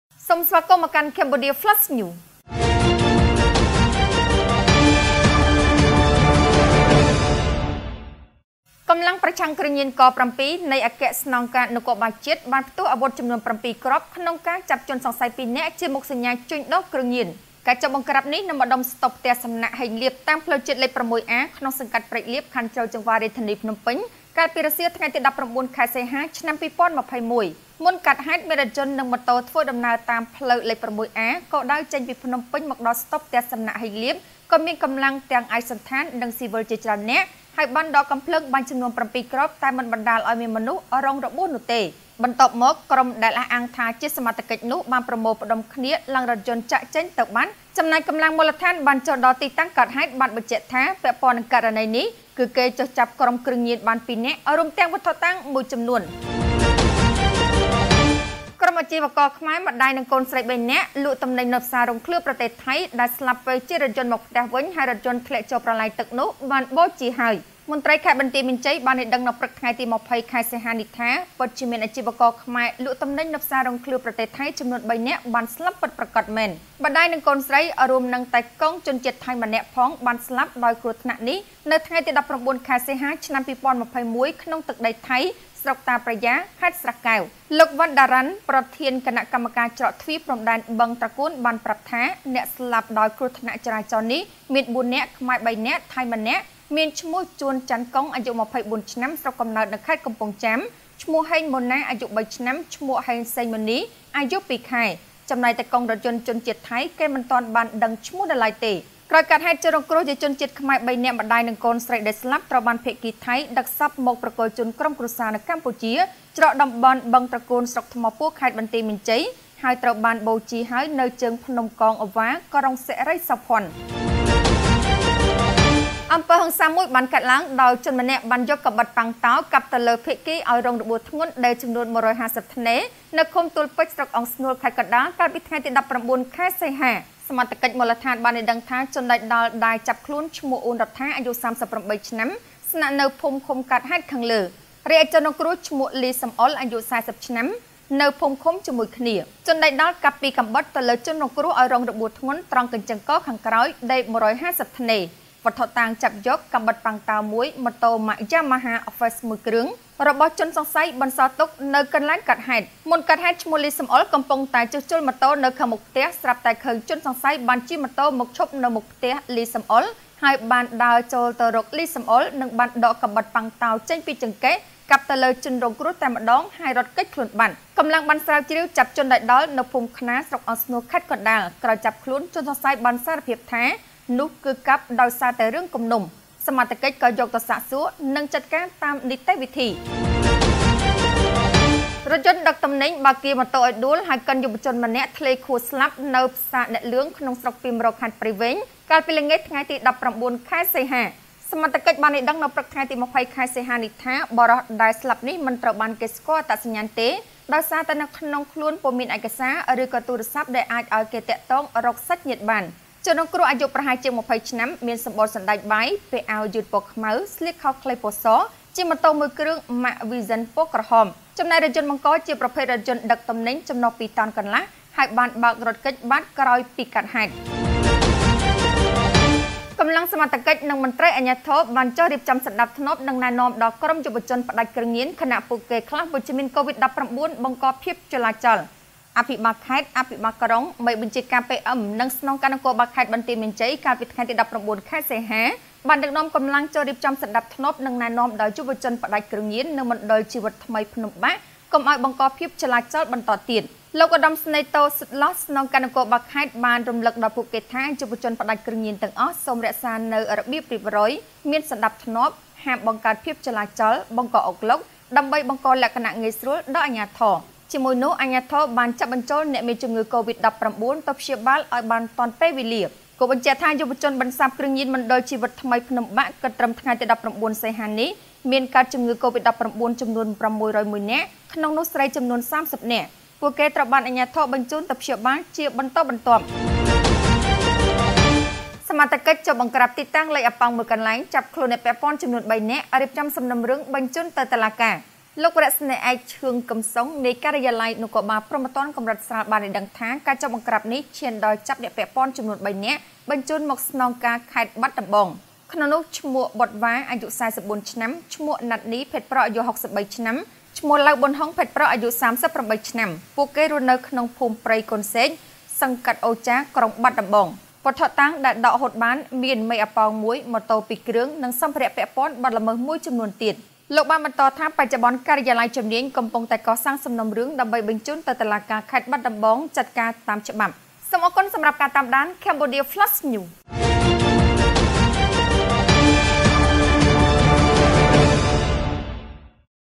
សមត្ថកិច្ចផ្ទុះអាវុធ៧គ្រាប់ចាប់ក្រុមគ្រឿងញៀន២នាក់ | CFN TV (ព័ត៌មានជាតិ) 20-8-21
ចំណងជើង៖
Cambodia Flash News (ព័ត៌មានជាតិ) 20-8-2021
-សមត្ថកិច្ចផ្ទុះអាវុធ៧គ្រាប់ចាប់ក្រុមគ្រឿងញៀន២នាក់
-ខ្មែរ៣នាក់ស្លាប់ដោយរថយន្តធ្លាក់ទឹកនៅថៃបូជានៅខ្មែរ
-ចាប់ជនយកកាំបិតប៉័ងតោកាប់គេឱ្យរបួសដេរ១៥០ថ្នេរ
-រថយន្តកៀរម៉ូតូឱ្យដួលកិនយុវជនម្នាក់ឱ្យធ្លាយខួរស្លាប់
-ចុះអប់រំក្រុមយុវជនផ្ដាច់គ្រឿងញៀនដែលមានកូវីដ-១៩
-សមត្ថកិច្ចបង្ក្រាបអ្នកលេងអាប៉ោងចាប់ខ្លួនមនុស្ស៣នាក់

THANK YOU FOR WATCH MY CHANNEL